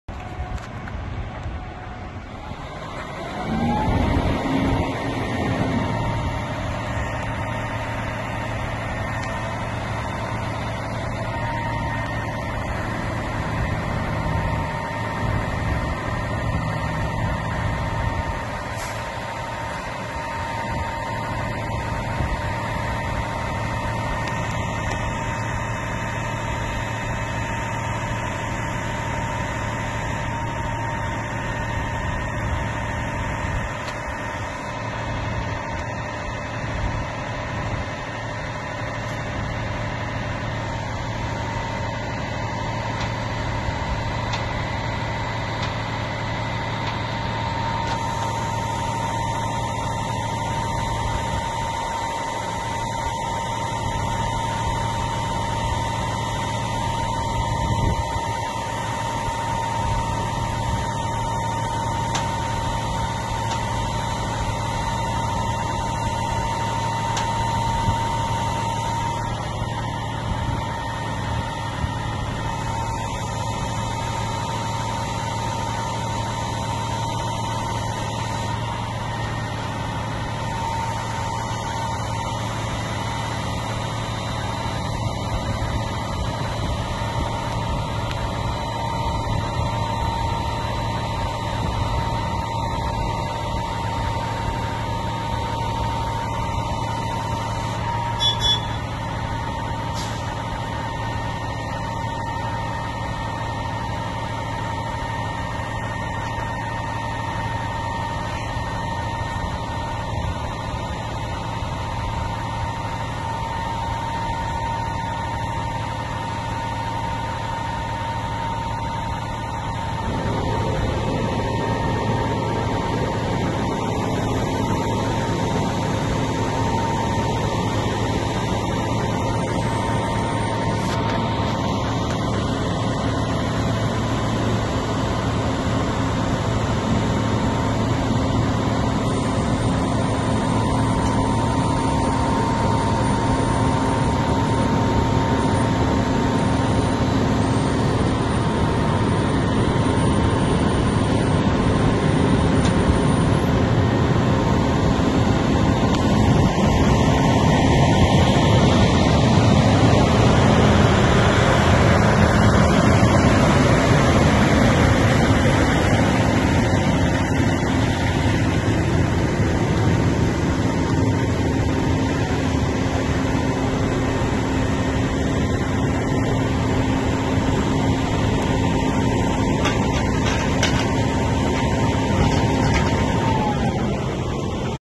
VIDEO Alberta C Box Set Up
C BOX Alberta- Being delivered and set up for installation